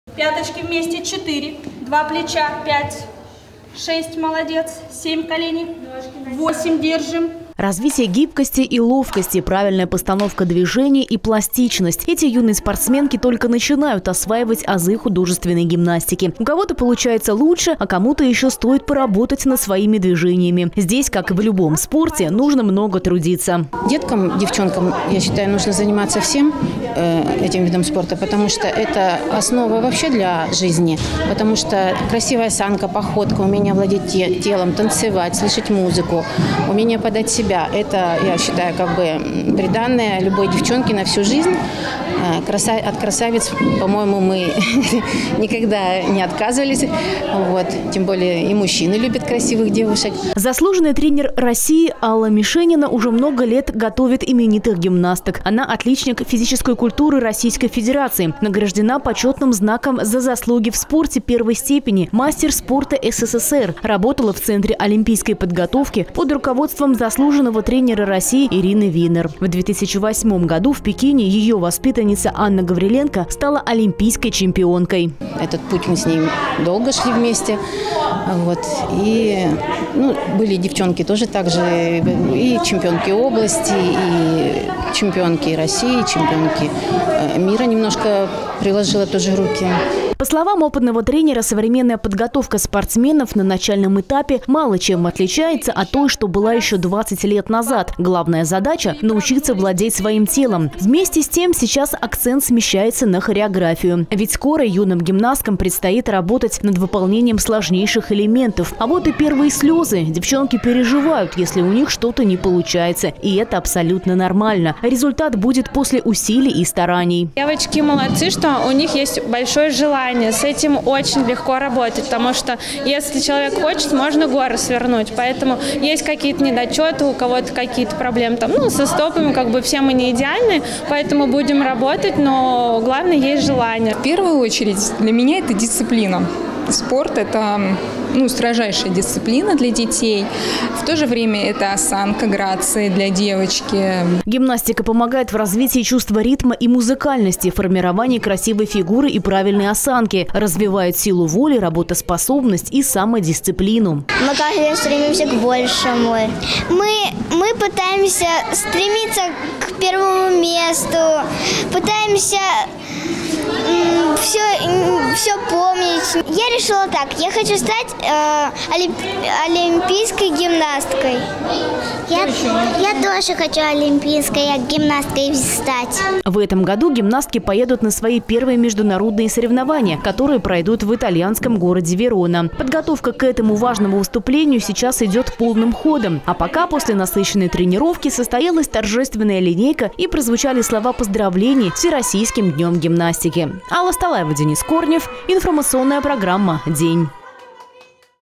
(12+) В Мытищах готовят олимпийских чемпионов
В октябре традиционно отмечается Всероссийский день гимнастики. Этот вид спорта завоевал популярность еще в прошлом веке. Но в календаре праздничных дат он появился только в 1999 году. Спортсменки нашей страны регулярно добивались в этом виде спорта значительных результатов, в том числе олимпийских медалей. Заслуженный тренер России Алла Мишенина точно знает, как воспитать будущих чемпионок, свою методику она сейчас отрабатывает на мытищинских спортсменках.